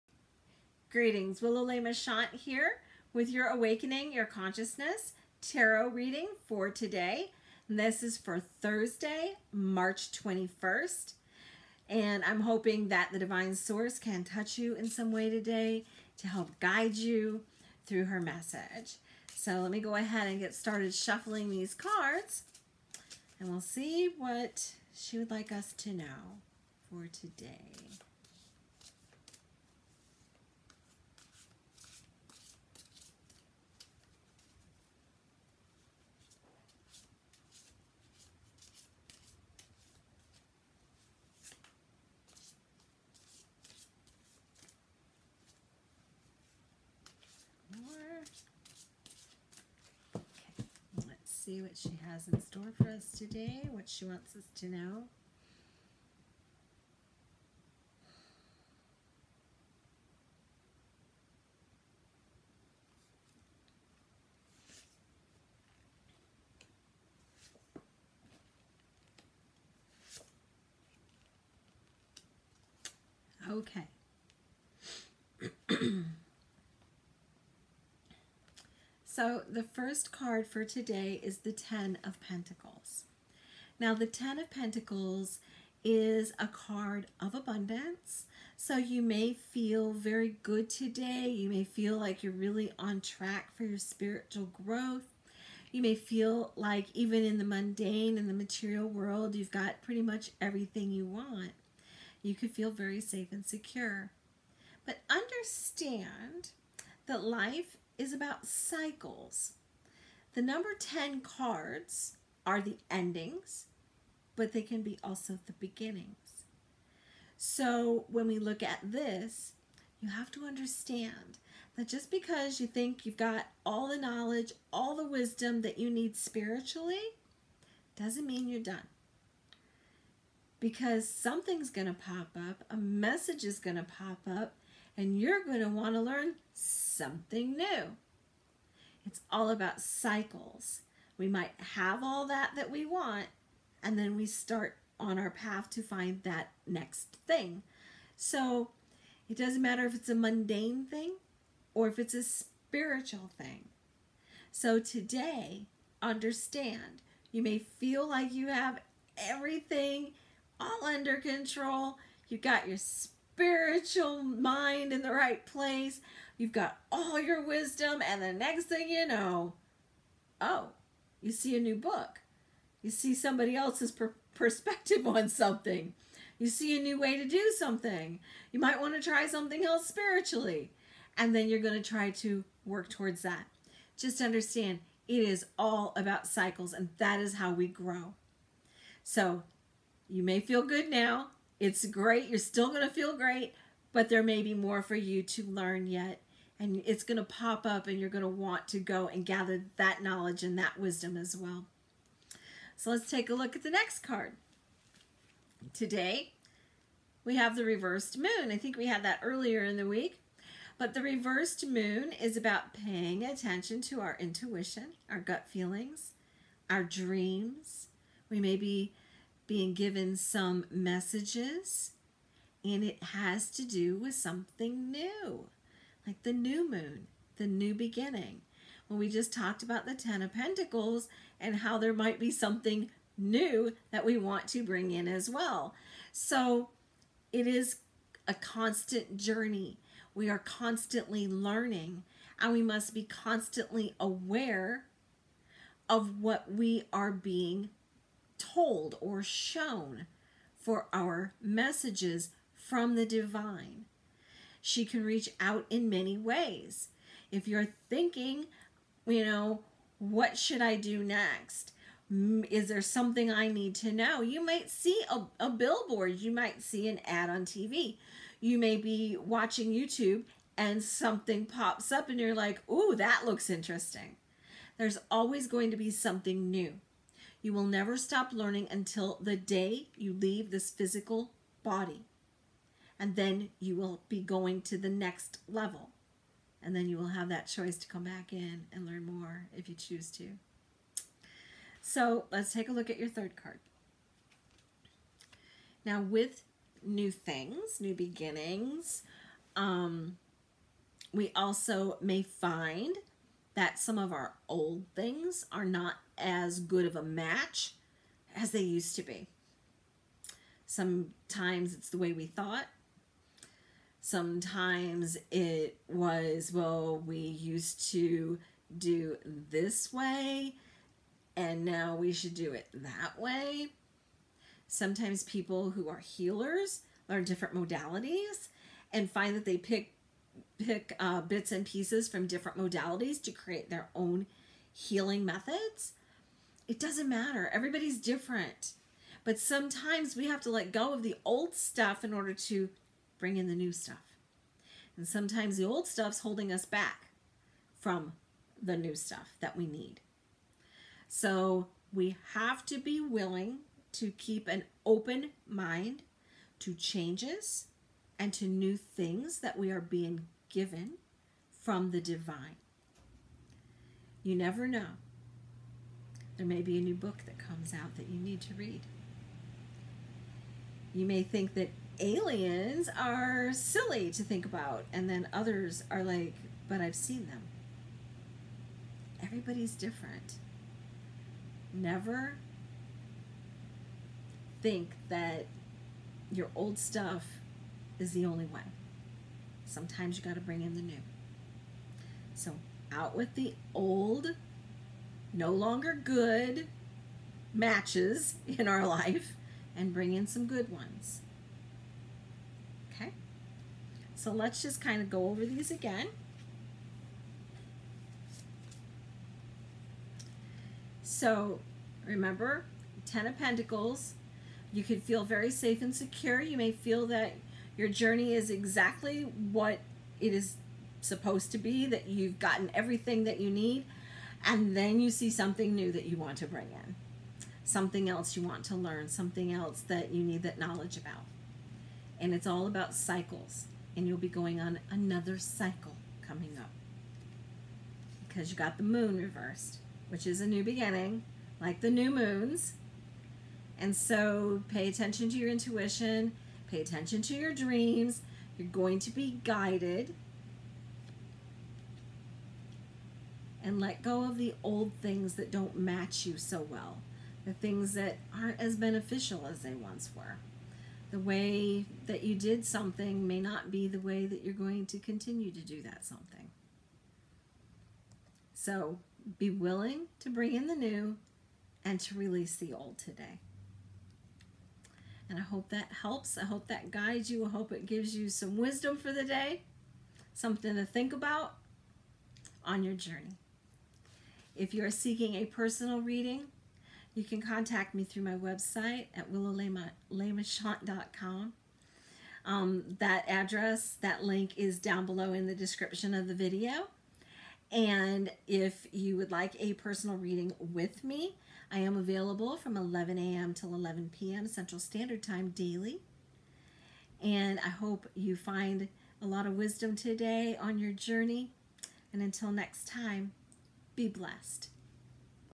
Awakening Your Consciousness - Reading for Thursday March 21st
Your daily reading to receive your message from the Divine Source to help guide your day.

Willowlemechant.com

@divinesourcenola and @willowlemechant

@DivineSourceNOLA and @WillowLeMechantSpiritualServices